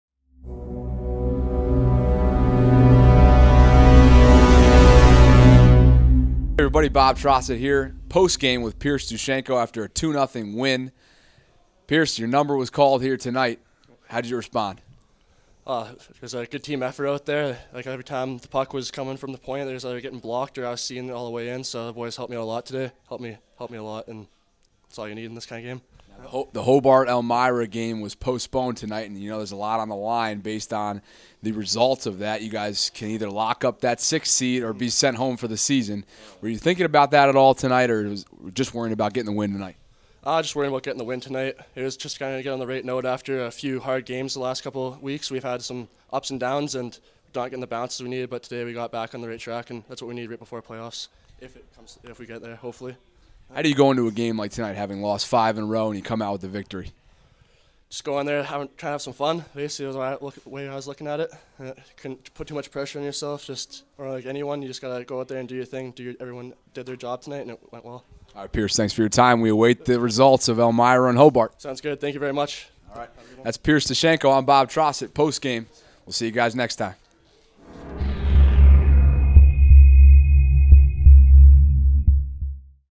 Men's Ice Hockey vs Lebanon Valley Postgame with Pierce Dushenko
Stevenson Freshman goaltender Pierce Dushenko talks about the team's play in their 2-0 shut-out of Lebanon Valley.